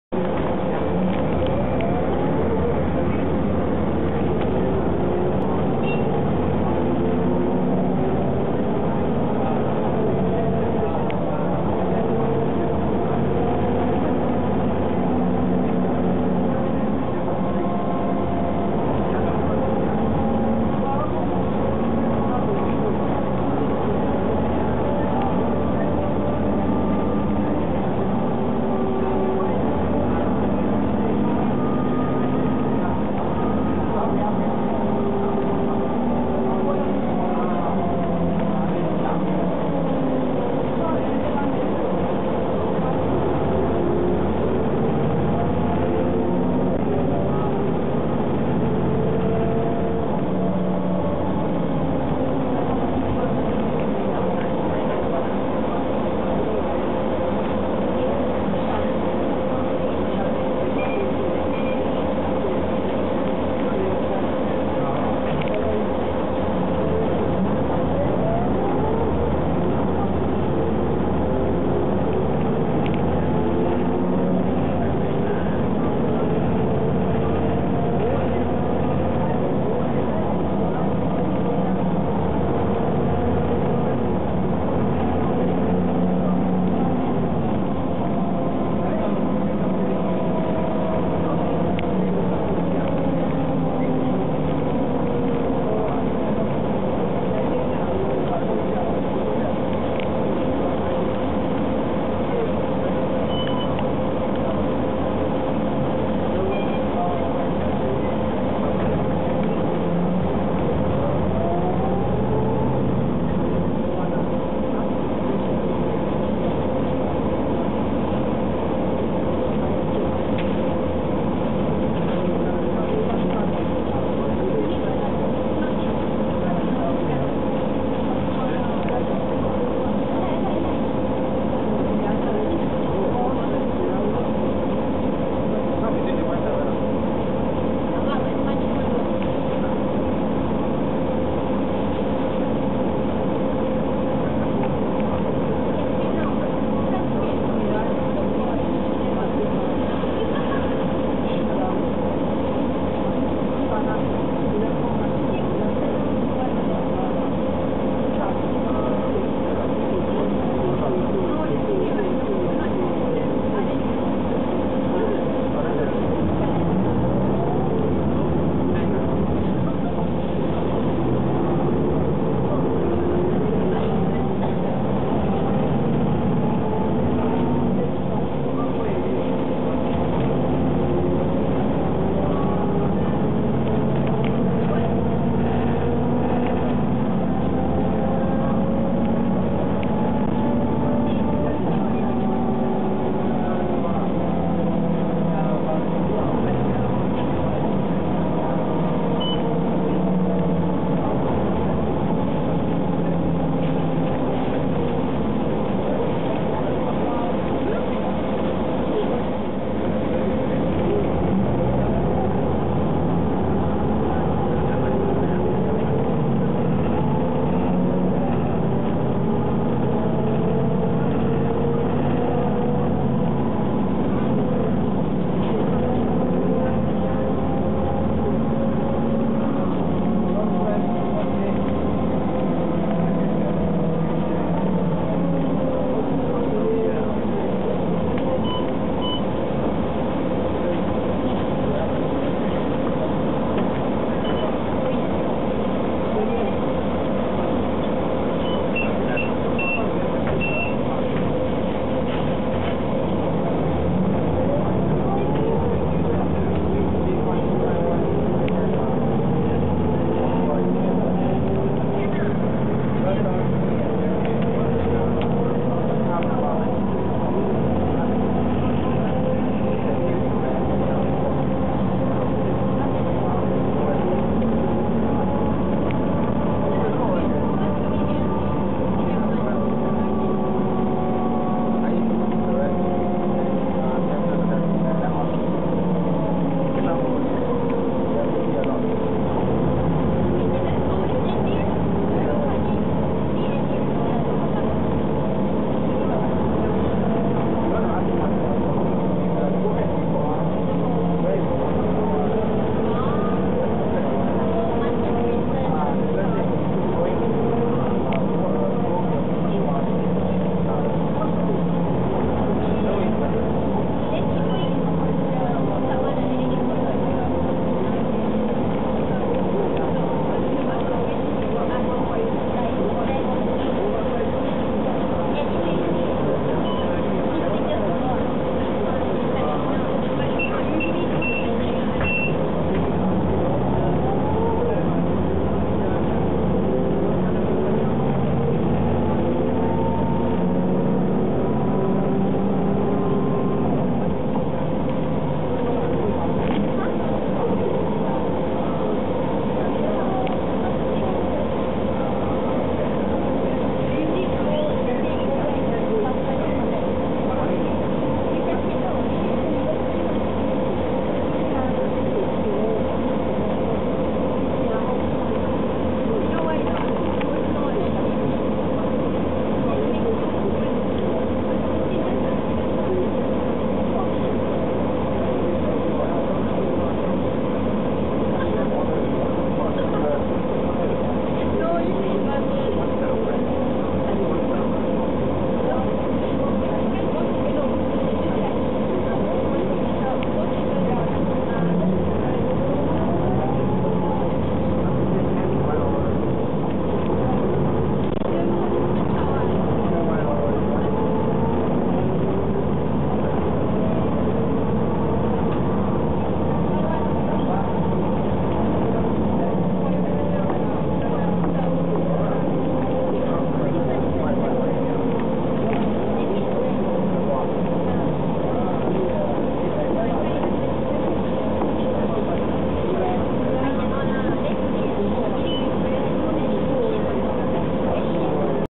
{Gearslip} SMRT: Mercedes-Benz O405 (TIB484U on Service 857) [30 Nov 2007]
Model: Mercedes-Benz O405
Bodywork: Hispano Carrocera
Country: Singapore
Vehicle Registration Plate No: TIB484U
Service: 857

Date of recording: 30 November 2007
Equipment used: Panasonic Lumix FZ-1